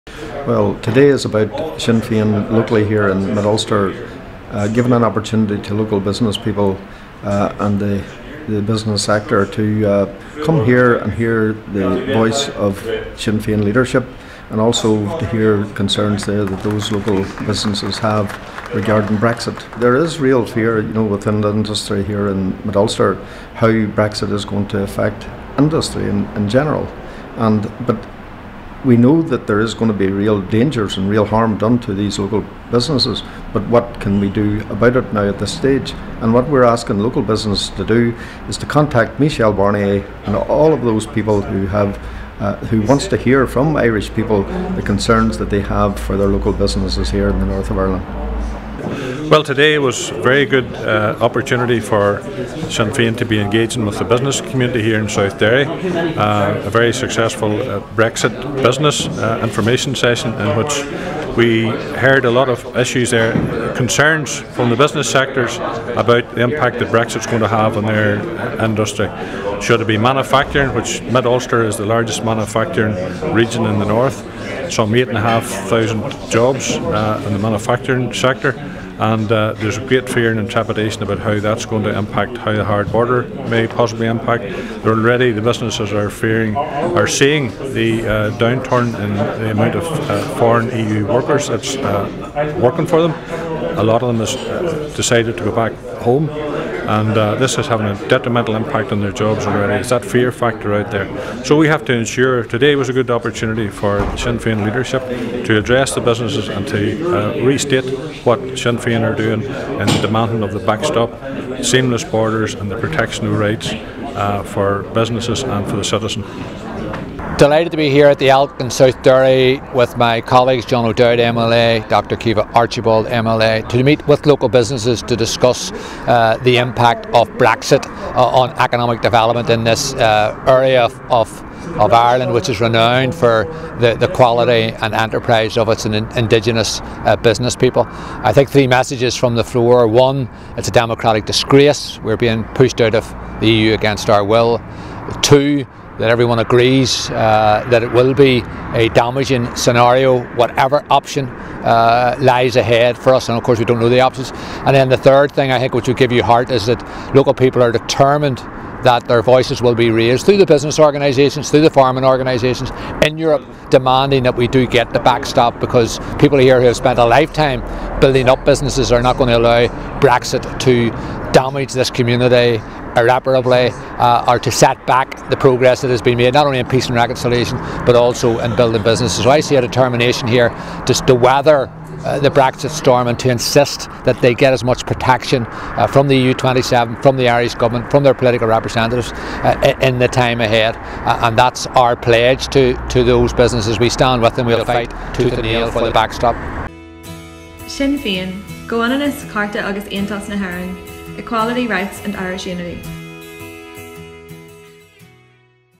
Sinn Féin will fight tooth and nail for Brexit backstop - Ó Muilleoir
Sinn Féin Brexit spokesperson Máirtín Ó Muilleoir tells a meeting of business leaders, farmers and community leaders in Mid Ulster that Sinn Féin will fight tooth and nail for the Brexit backstop to defend their interests and rights.